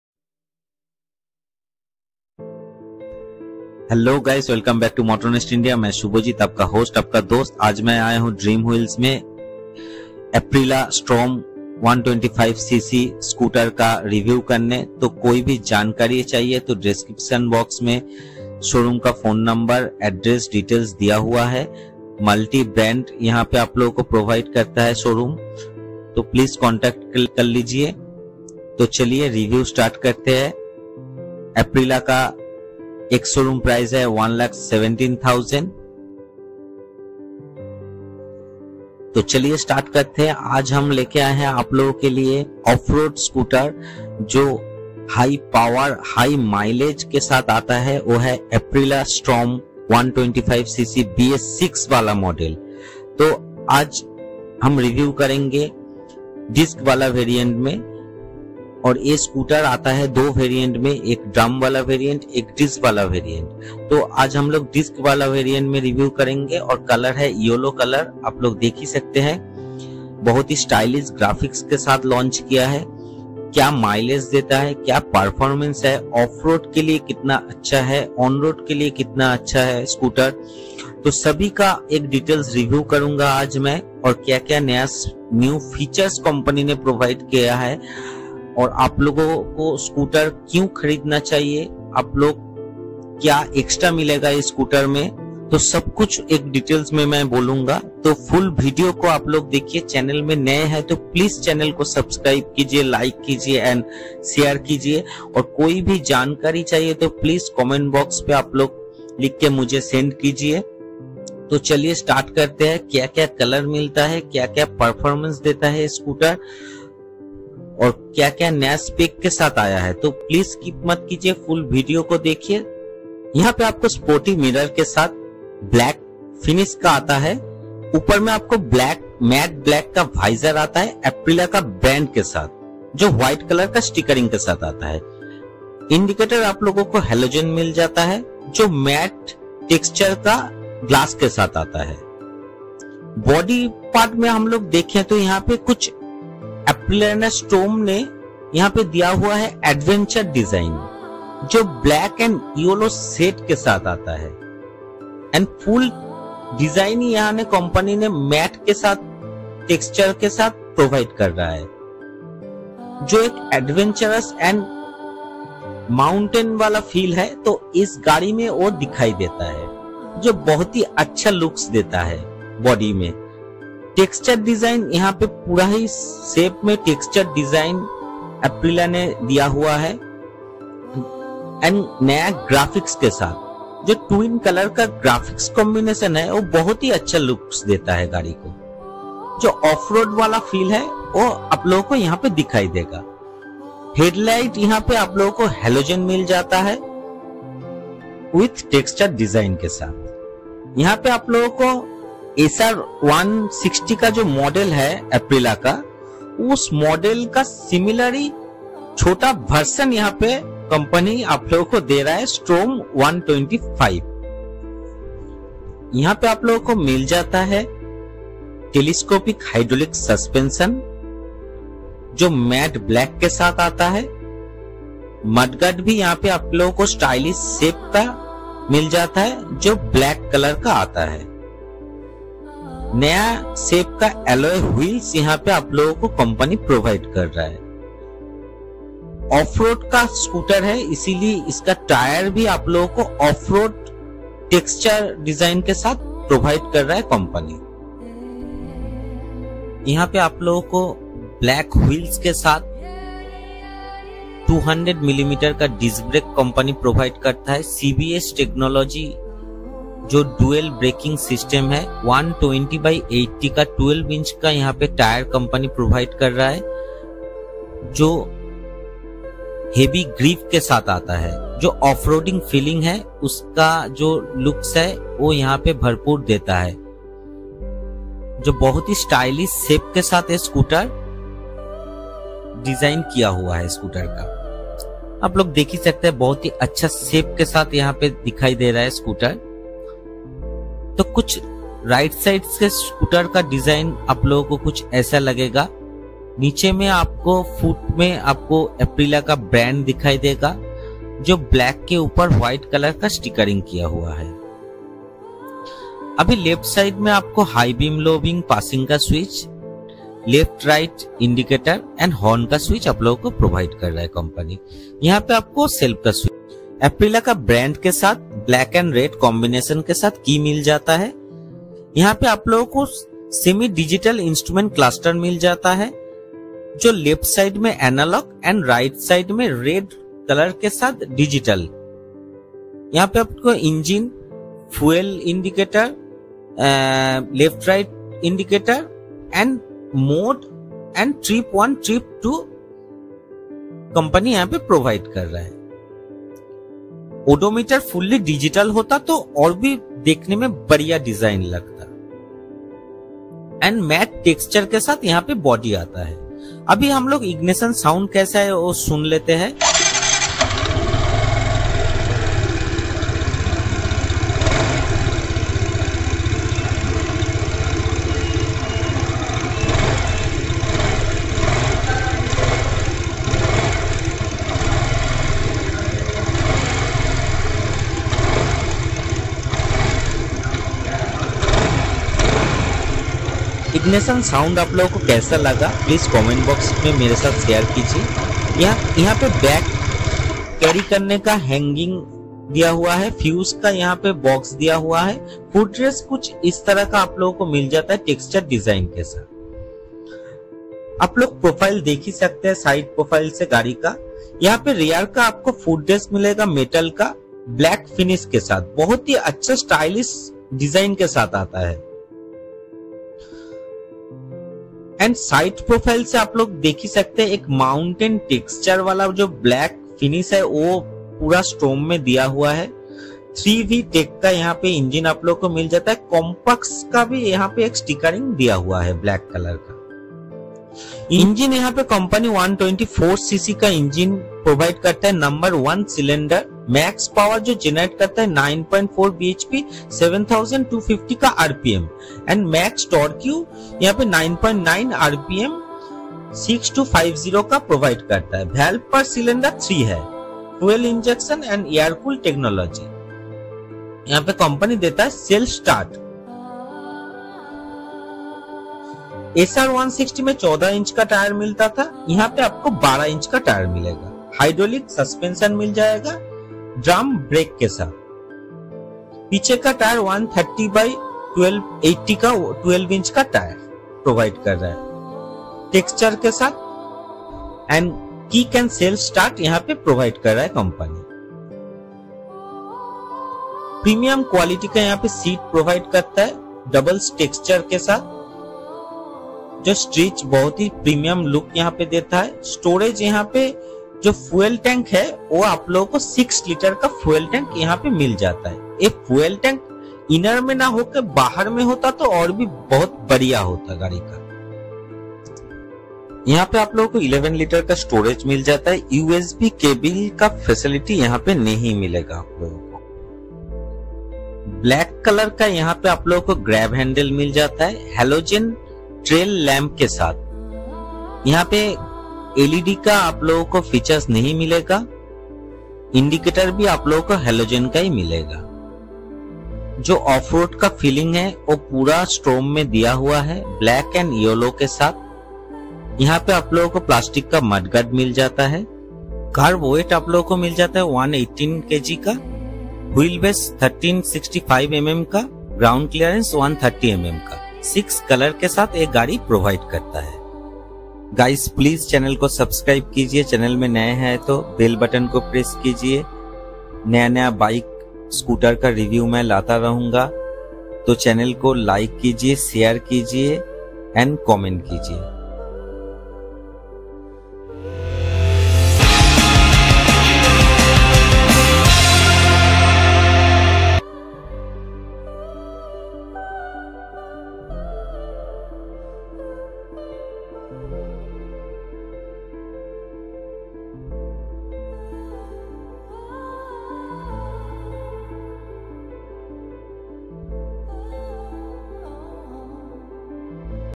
New Aprilia STORM 125 BS6 Detailed Hindi Review | Feature | Price | Best 125cc on/off Road Scooter
Dream Wheels - We Deal in All Types of Two Wheelers

Address: Salap More, Salap, Amta Road, NH-6, Howrah, West Bengal 711403

One of the most affordable racing and performance scooters of India.
Even though the Aprilia Storm 125 is based on the Aprilia SR 125, there are some minor differences between both scooters. The colour options of Aprilia Storm 125 are Red, White, Black, Yellow, Blue.. Also, to keep costs in check, the Aprilia Storm 125 gets a single halogen headlamp as opposed to the dual-halogen unit seen on the SR 125. The major change, however, is the tyre setup. The Aprilia Storm 125 features 12 inch dual-purpose tyres measuring 120/80-section at the front and 130/80-section at the rear. The SR 125, on the other hand, gets 14-inch wheels at both ends. The suspension setup is the same as its sibling: telescopic fork up front and a monoshock unit at the rear. The Aprilia Storm 125 misses out on the front disc brake which is offered on the Aprilia SR 125 and makes do with drum units at both ends. In terms of rivals, it goes up against the TVS NTorq 125 xt, Hero Maestro Edge 125, Yamaha ZR 125 and Suzuki burgman 125.

Aprilia Storm 125 Summary:
Engine Capacity: 124.45 cc
Mileage: 40 kmpl
Kerb Weight: 118 kg
Seat Height: 755 mm
Fuel Tank Capacity: 6 litres
Max Power: 9.78 bhp